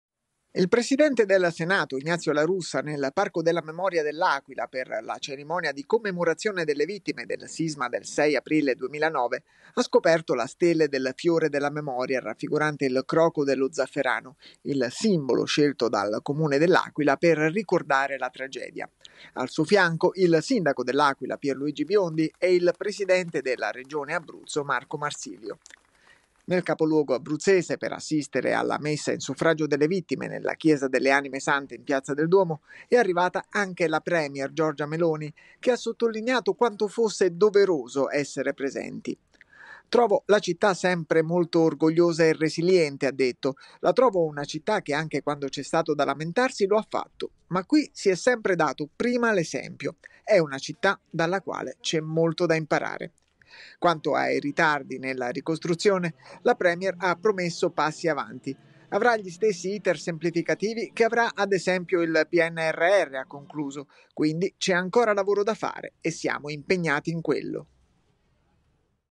Terremoto, La Russa a L'Aquila scopre stele "fiore della memoria"
L'Aquila, 5 apr. (askanews) - Il presidente del Senato, Ignazio La Russa, nel parco della memoria dell'Aquila per la cerimonia di commemorazione delle vittime del sisma del 6 aprile 2009, ha scoperto la stele del "fiore della memoria", raffigurante il croco dello zafferano, il simbolo scelto dal Comune dell'Aquila per ricordare la tragedia.
Al suo fianco, il sindaco dell'Aquila, Pierluigi Biondi e il presidente della regione Abruzzo, Marco Marsilio.
Nel capoluogo abruzzese per assistitere alla messa in suffragio delle vittime nella Chiesa delle anime sante in piazza del Duomo, è arrivata anche la premier, Giorgia Meloni che ha sottolineato quanto fosse doveroso essere presenti.
"Trovo la città sempre molto orgogliosa e resiliente - ha detto - la trovo una città che anche quando c'è stato da lamentarsi lo ha fatto ma qui si è sempre dato prima l'esempio. È una città dalla quale c'è molto da imparare".
Quanto ai ritardi nella ricostruzione, la premier ha promesso passi avanti.
"Avrà gli stessi iter semplificativi che avrà ad esempio il Pnrr - ha concluso - quindi c'è ancora lavoro da fare e siamo impegnati in quello".